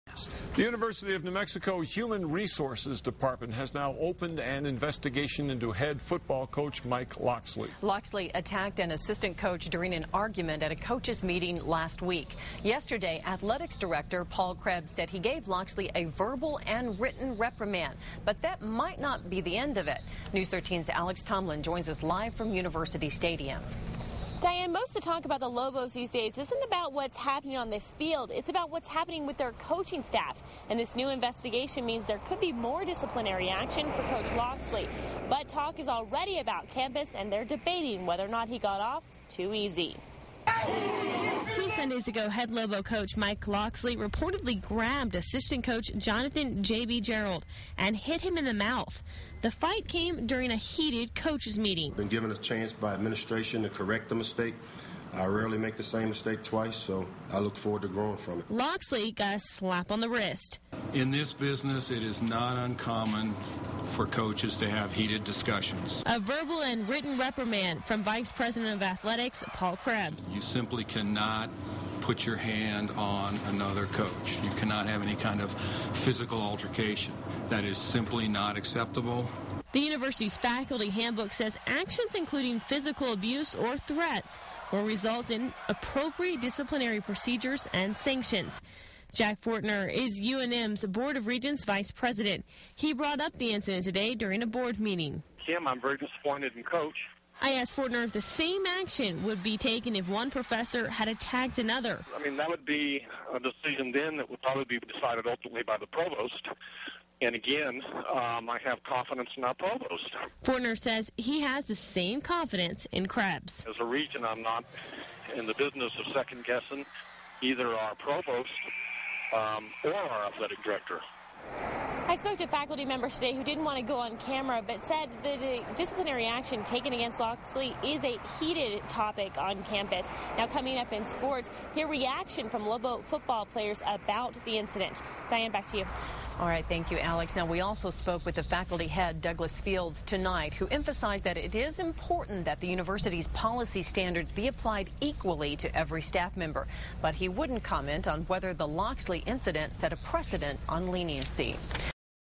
Coach's altercation ordeal continues
A reprimand from the athletic director may not end the trouble for University of New Mexico football Head Coach Mike Locksley after he admitted punching one of his assistants.